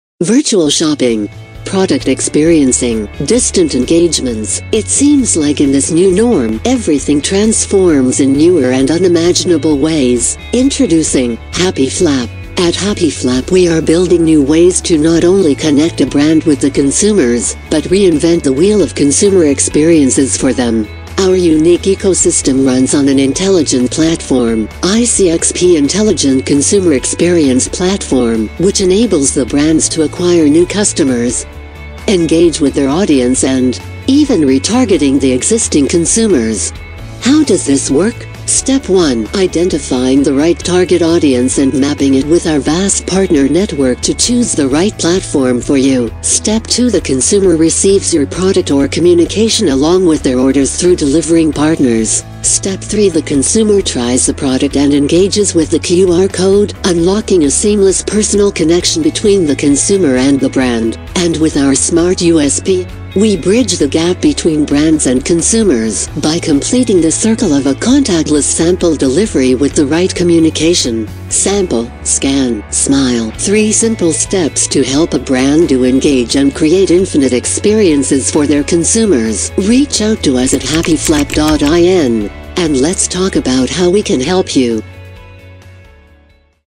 Happy Flap - An Intelligent Sampling Platform
Introducing
HappyFlap!
At Happyflap we are building new ways to not only connect a brand with the consumers but re-invent the wheel of consumer experiences for them.
Our unique ecosystem runs on an intelligent Platform
ICXP = Intelligent Consumer Experience Platform
which enables the brands to Acquire new customers,
Engage with their audience and even Re-targeting the existing consumers.

HOW DOES THIS WORK?
Step 1 – Identifying the right TG and mapping it with our vast partner network to choose the right platform for you.
Step 2 - The consumer receives your product or communication along with their orders through delivering partners
Step 3 - The consumer tries the product and engages with the QR Code, unlocking a seamless personal connection between the consumer and the brand

And with our S.M.A.R.T USPs
 Smart consumer acquisition through trials
 Multi channel consumer reach
 Actionable data analytics
 Retargeting possibilities with intelligent database
 Transfer of acquired data to existing client CRM/CDP

We brigde the gap between brands and consumers by completing the circle of a contactless sample delivery with the right communication!

SAMPLE SCAN SMILE
3 simple steps to help a brand to engage and create infinite experiences for their consumers